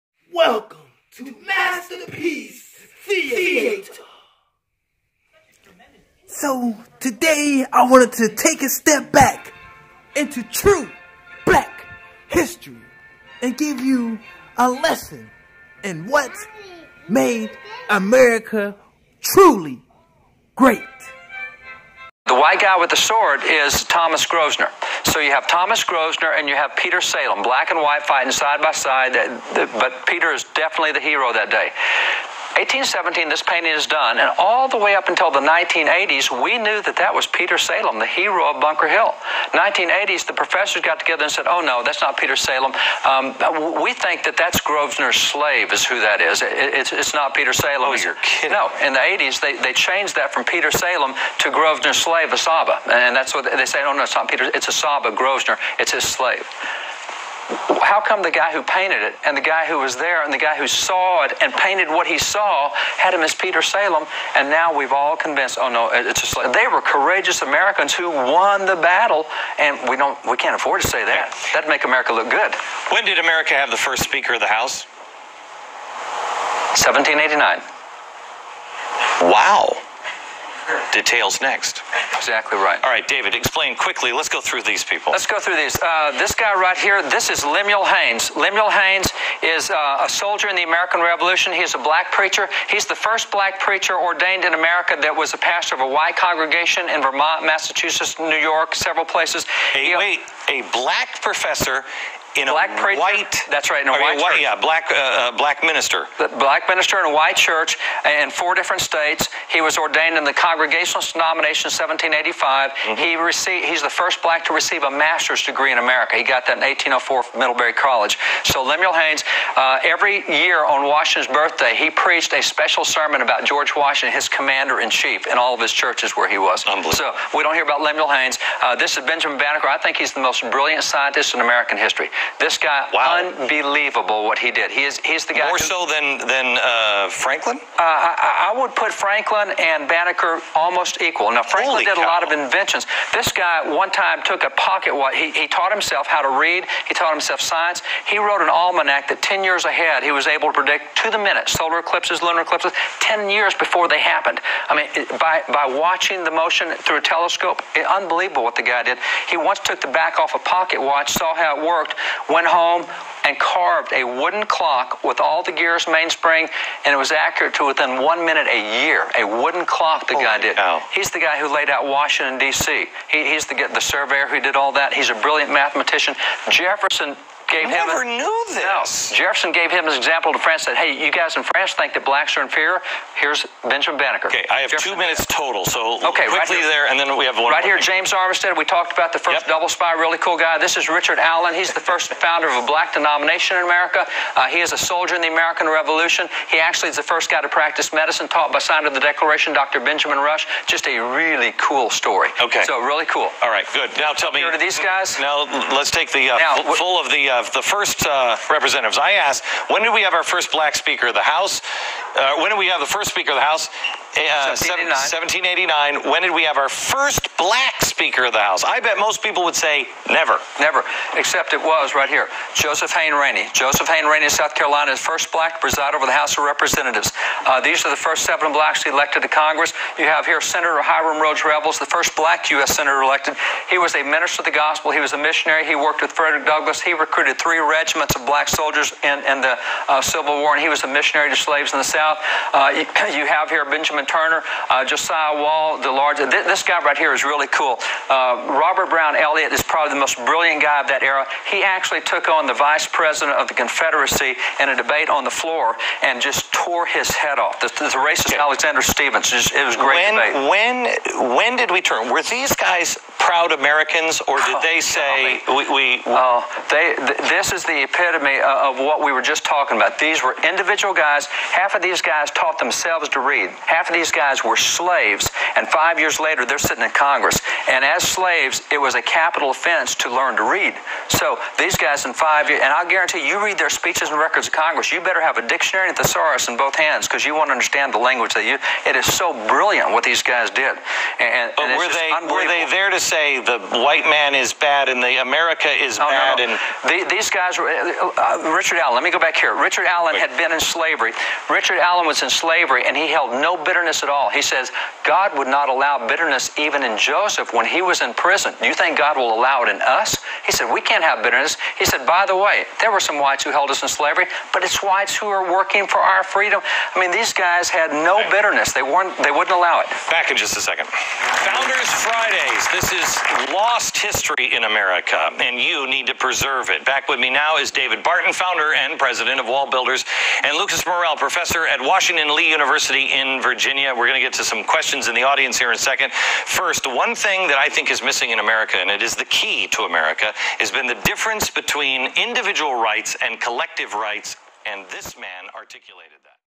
"Black Founding Father's" (Black MACGYVER Sheds light On the Topic During BLACK HISTORY Month!)
Raising awareness this black History Month. In order for young African American youths to see us do more than what is being perpetuated in today's society.